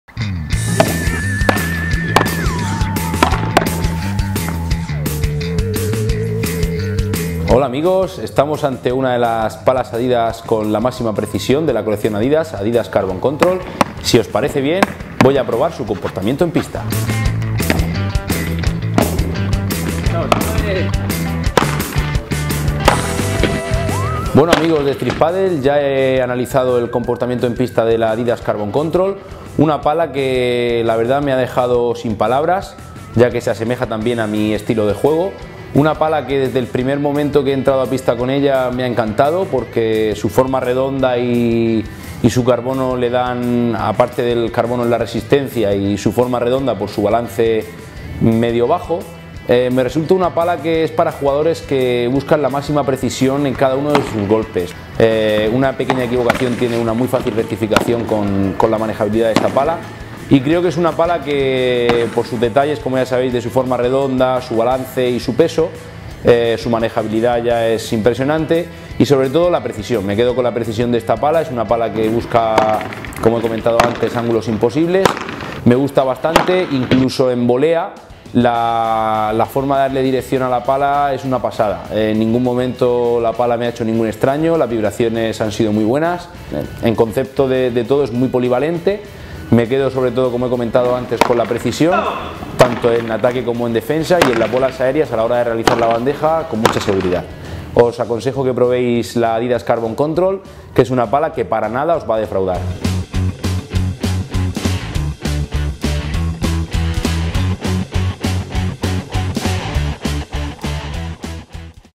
Análisis en pista pala de pádel Adidas Carbon Control
Analizamos el comportamiento en pista de la pala de pádel Adidas Carbon Control, con forma redonda, perfil de 38mm y con un balance relativamente bajo, haciendo de ella una de las mejores palas de pádel de control de Adidas este año.

¿Qué os parece esta Adidas Carbon Control? ¡Leemos vuestras opiniones!